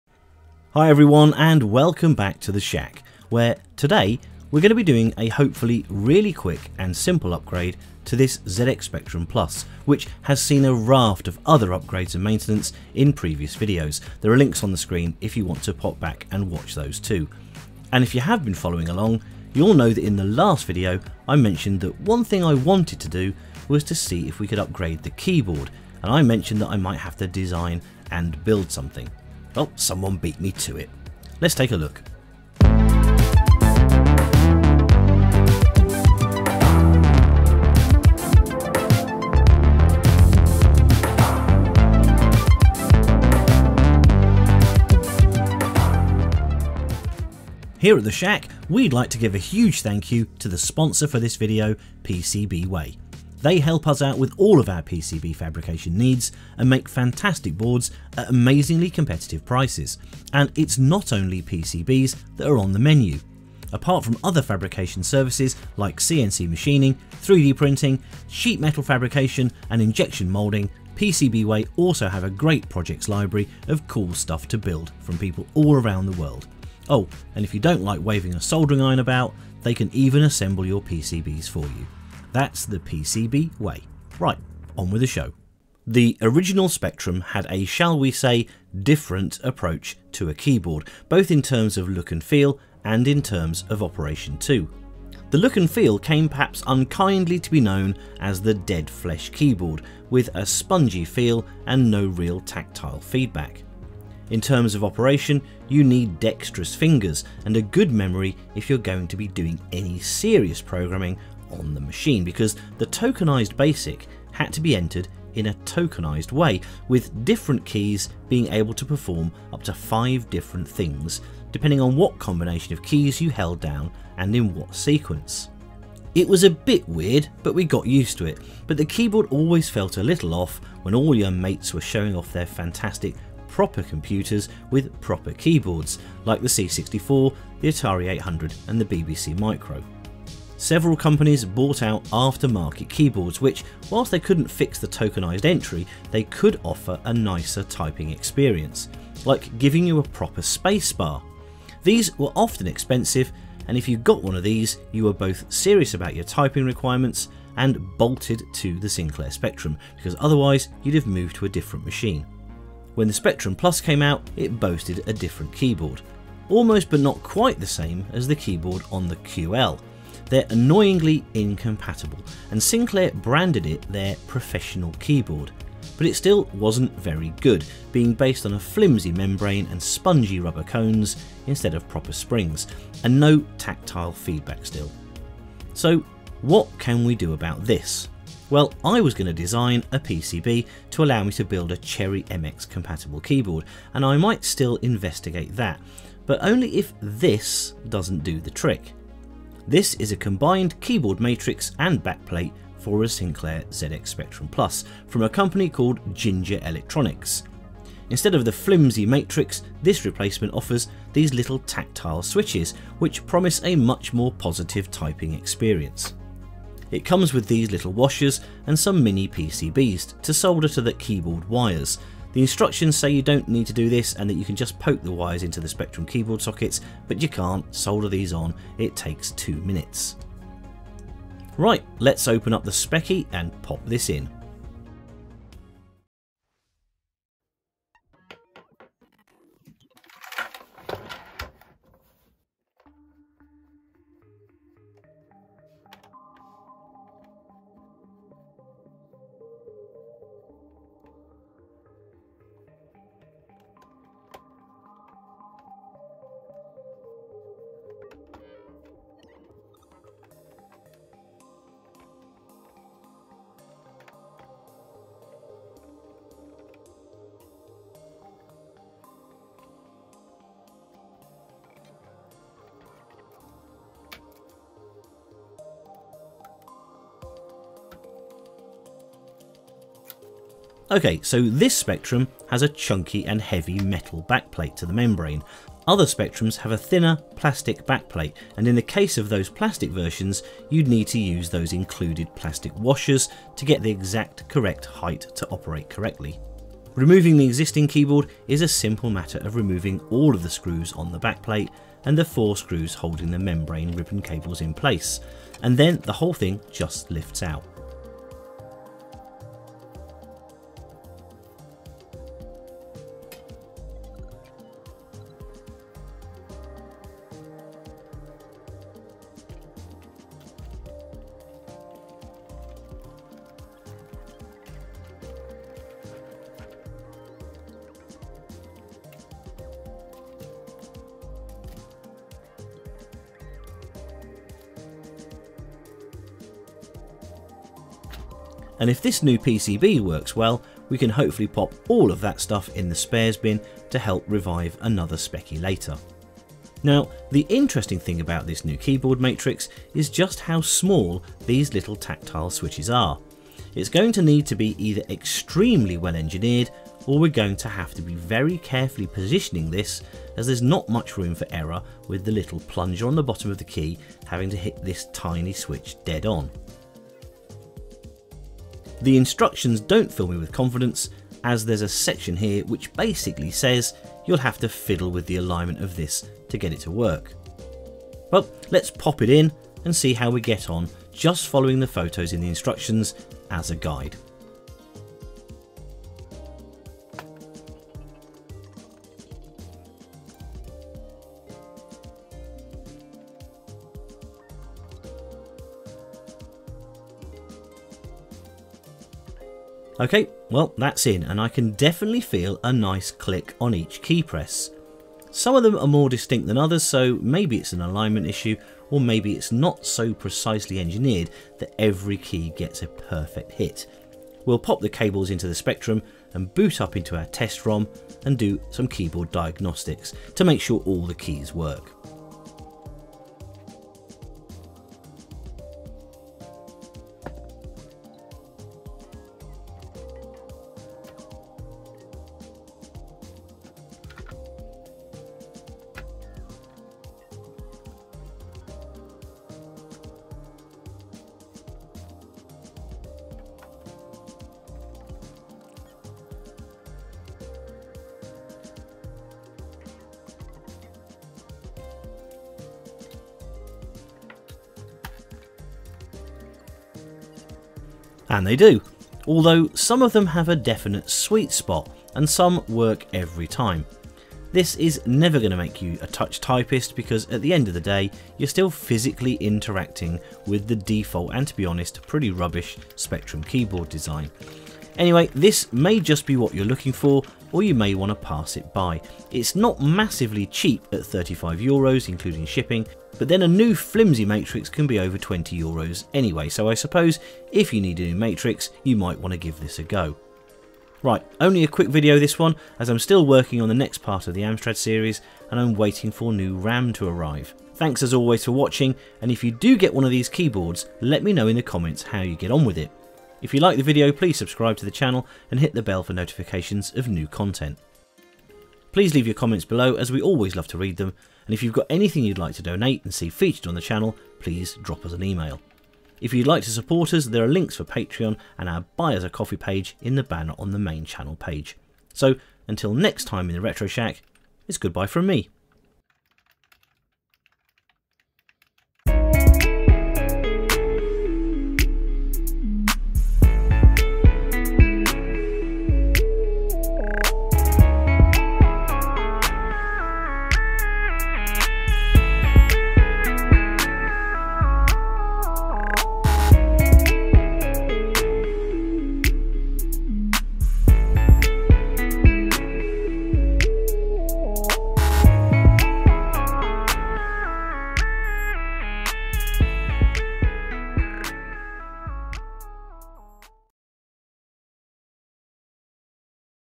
Is this Spectrum keyboard matrix the answer to Mavis Beacon's dreams?
This replacement keyboard matrix for the ZX Spectrum Plus promises a tactile feel and a new typing experience, but is it any good?  - PCBs for as little as $5!

Timestamps:
00:00 Intro
01:40 Dead Flesh Keyboards!
02:25 Alternate keyboards
04:00 Installation
07:19 Testing